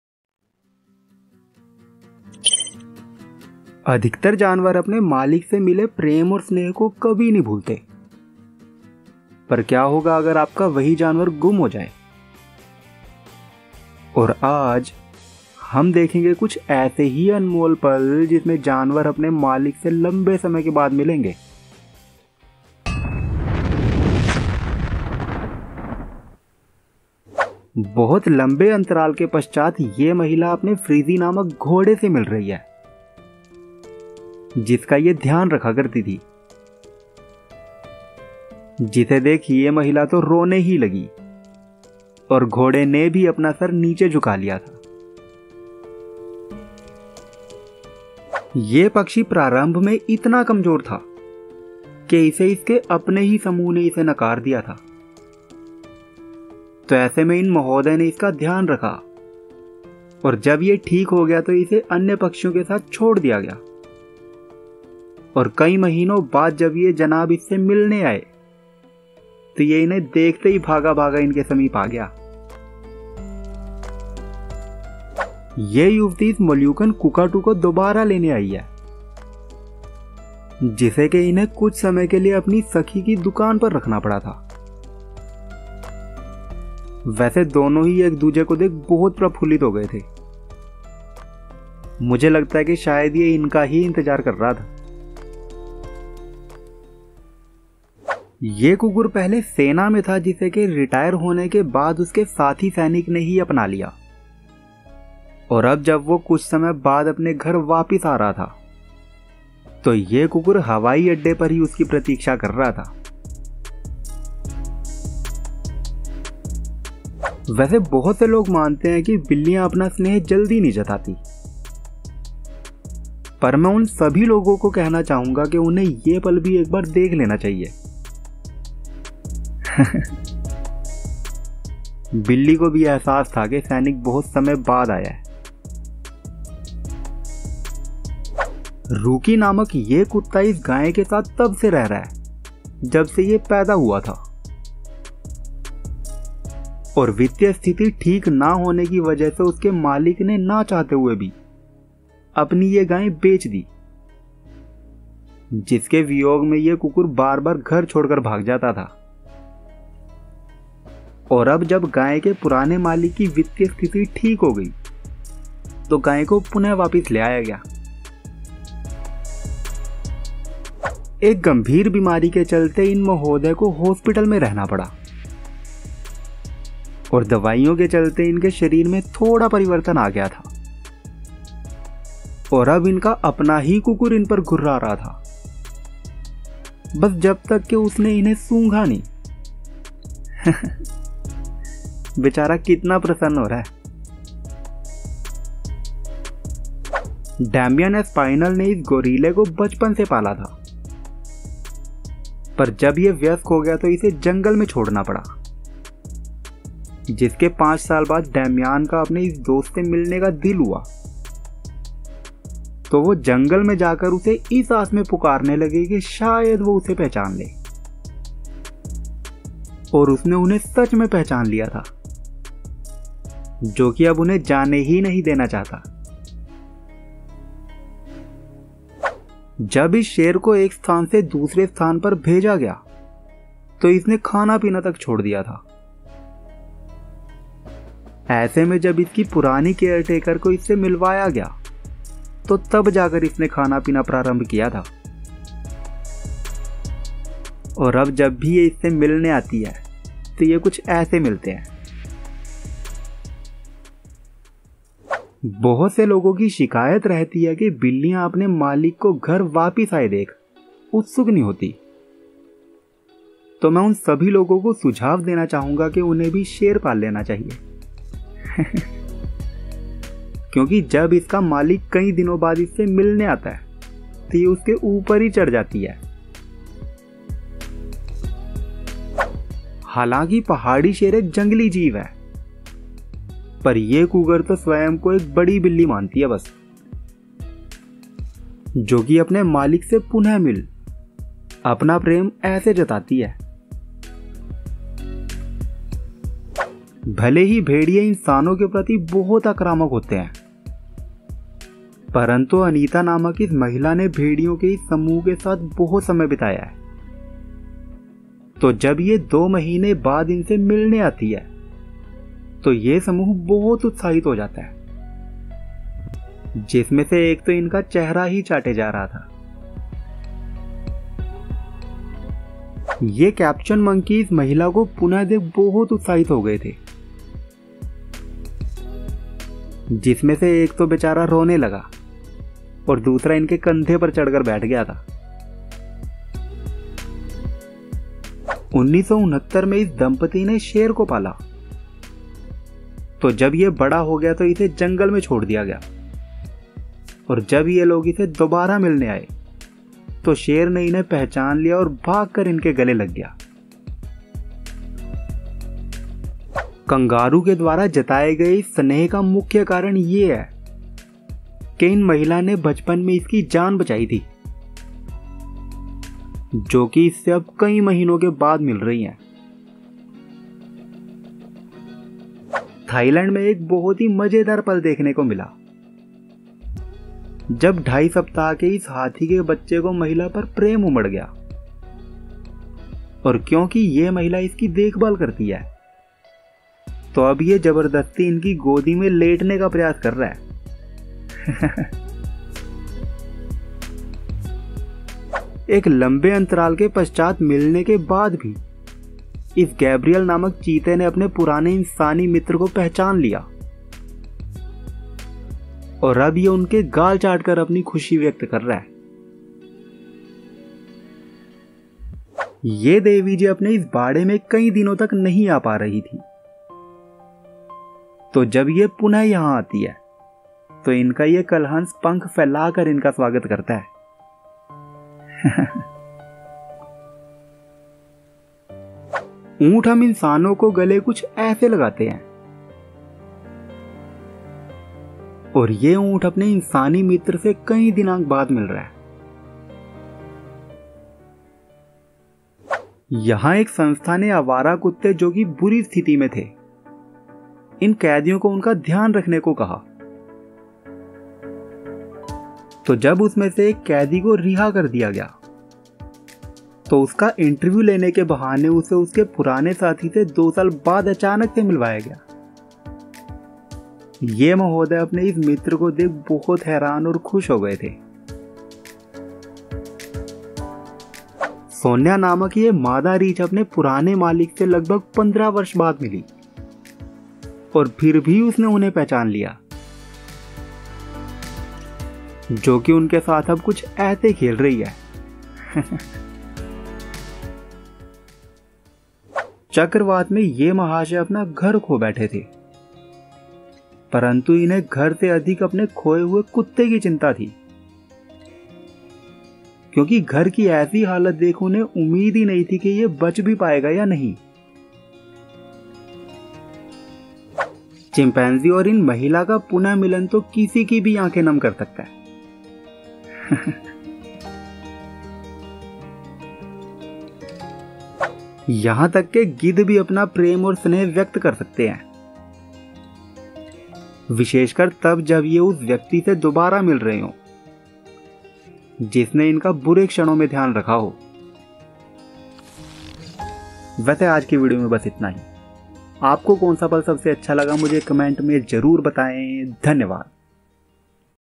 सालों बाद जानवर जब दोबारा मिले | 30 Animals Reunited With Owners After Years !
IMPROVED QUALITY
Most animals never truly forget their owners' love and care. If your pet's eyes fill with sadness just from you going to work, then imagine what it’s like for animals that have been separated from their owners for years! So, Today IKKNSH FACTS going to see some heartwarming moments of domestic & wild Animals Reuniting with Their owners  Ever Caught On Camera!